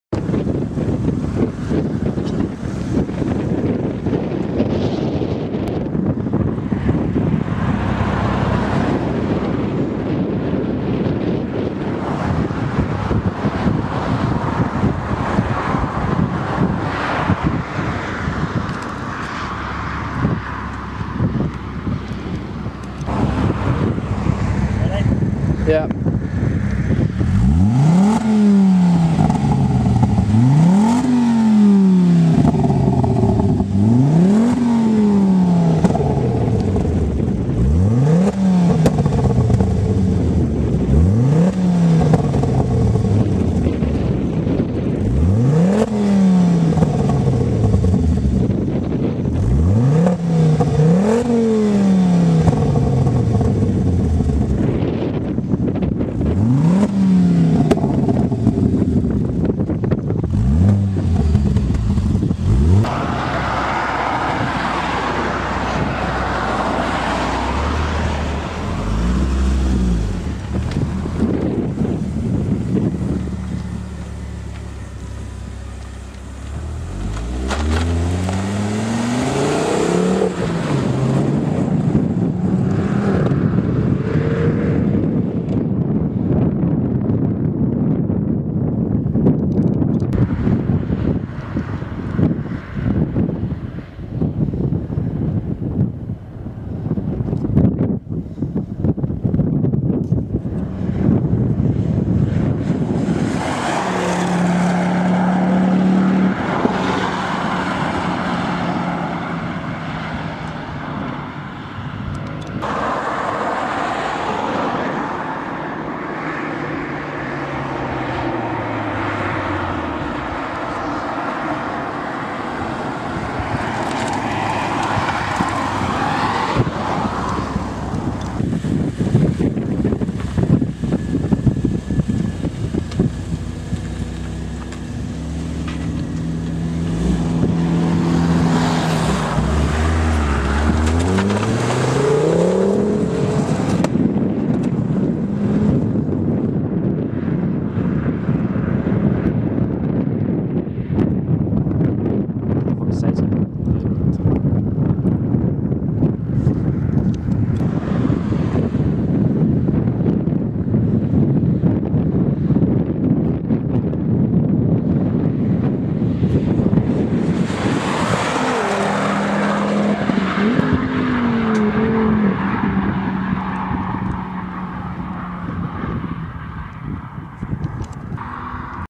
Mountune Focus RS sounds
An MP350 with exhaust, and an RS with Mountune exhaust play around.